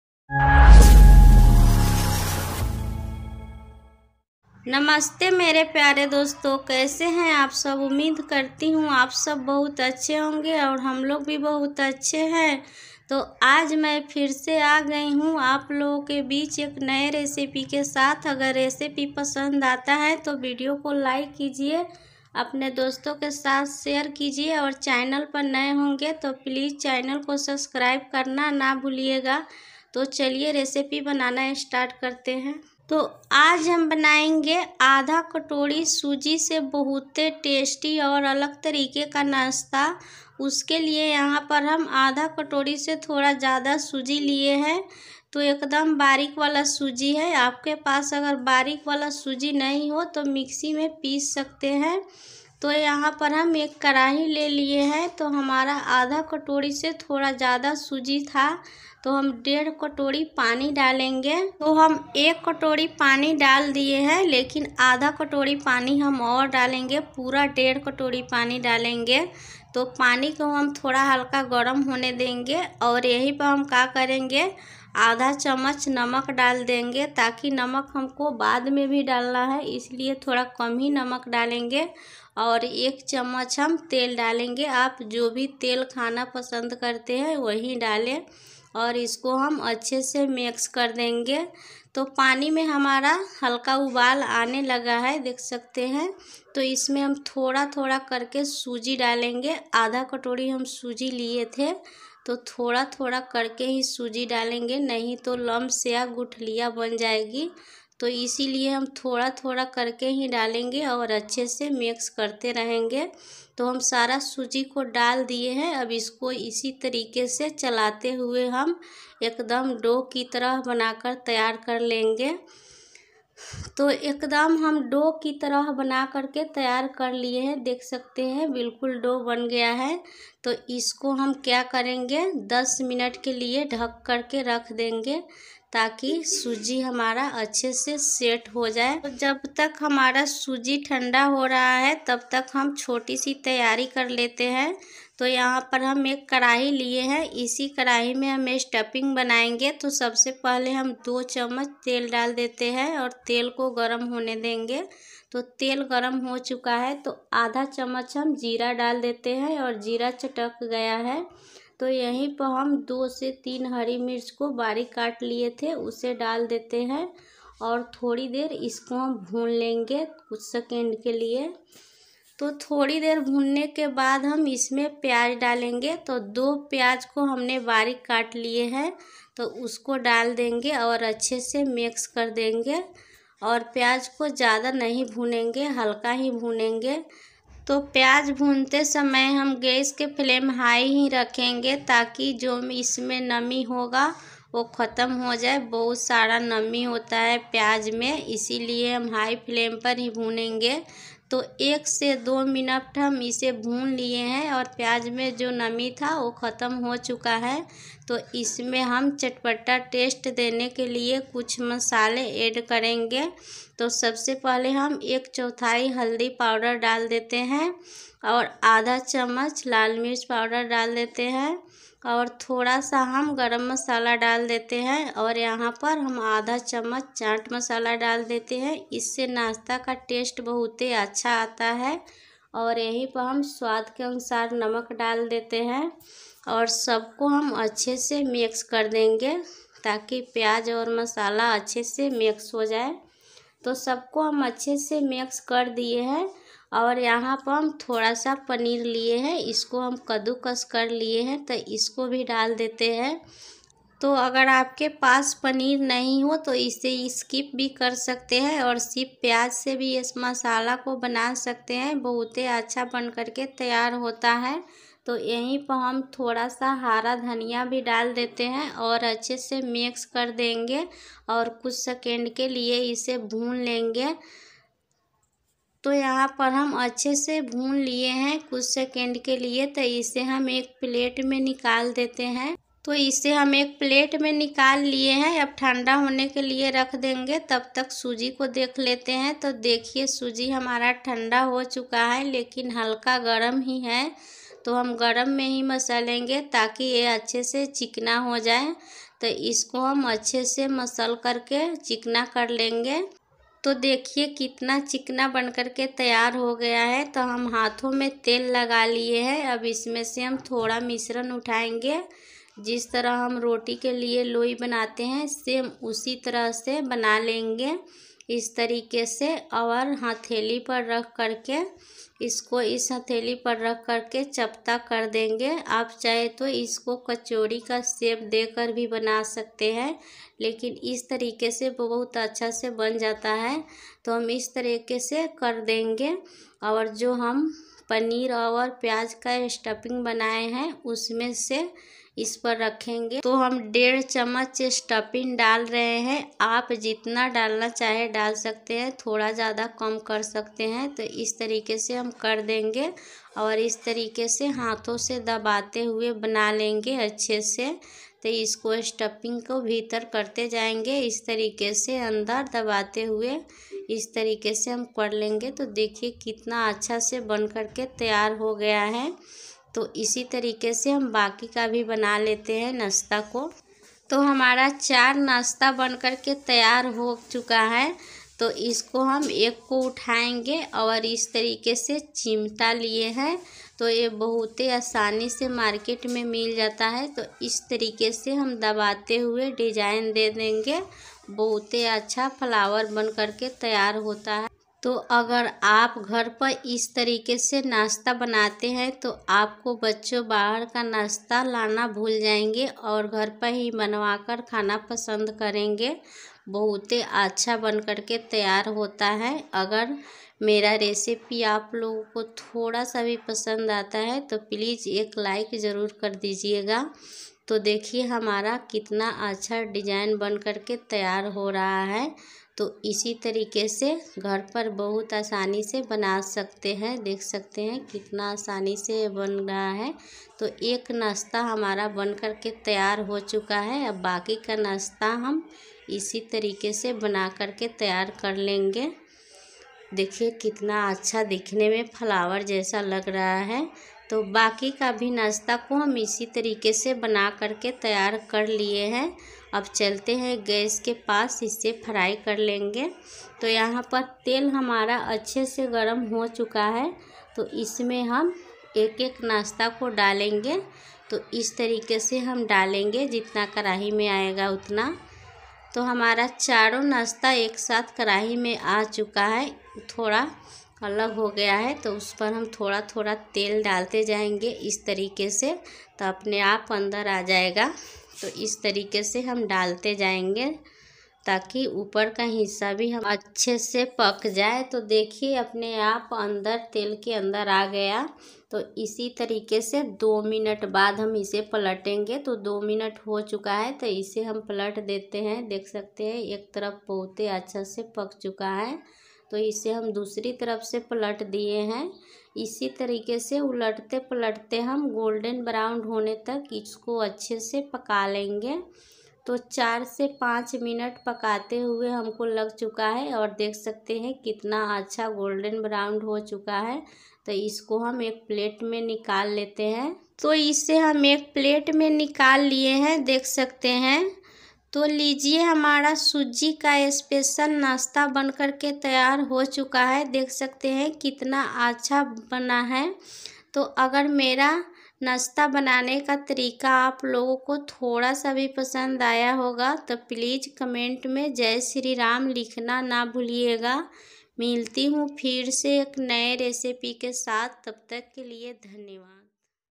कहीं ना देखी होगी सूजी का ऐसी अनोखी नाश्ता कि आपके बच्चे देखते ही खुश हो जाएंगे
सूजी के हलवा
 सूजी के बर्फी
सूजी के ठेकुआ
सूजी के मिठाई
सूजी के गुलाब जामुन
सूजी के पेड़ा
सूजी के नाश्ता
आधा कटोरी सूजी
 आधा चम्मच  नमक
 एक चम्मच सरसों का तेल
आधा चम्मच जीरा
दो प्याज
 दो हरी मिर्च
आधा चम्मच लाल मिर्च
 आधा छोटा चम्मच हल्दी
थोड़ा सा गरम मसाला
आधा चम्मच चैट
बड़ी कटा हुआ हरा धनिया